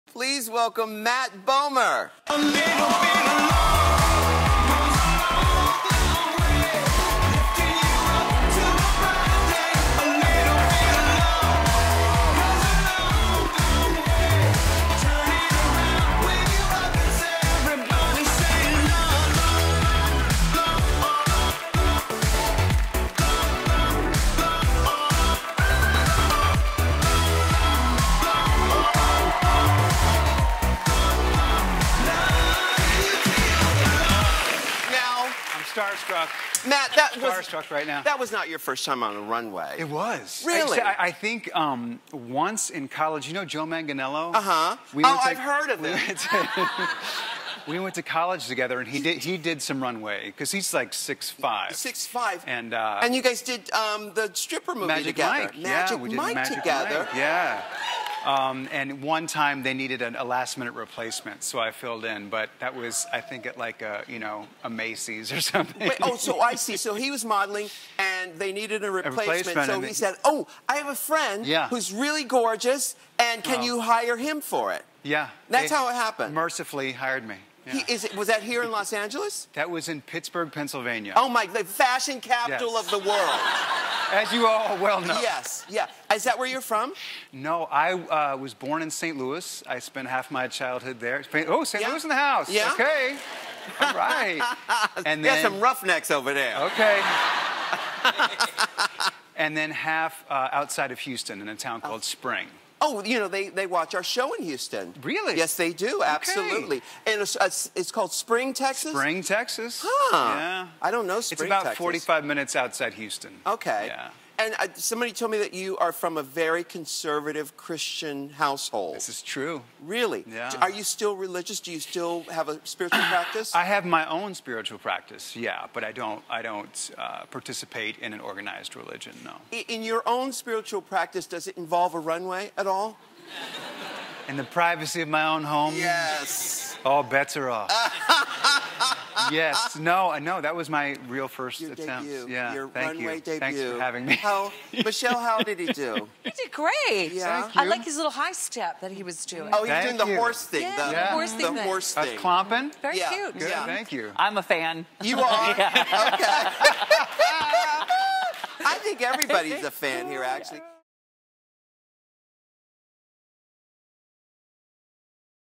Matt Bomer on Going to College and Modeling with Joe Manganiello
Matt Bomer reveals that Joe Manganiello gave him his first modeling job in college.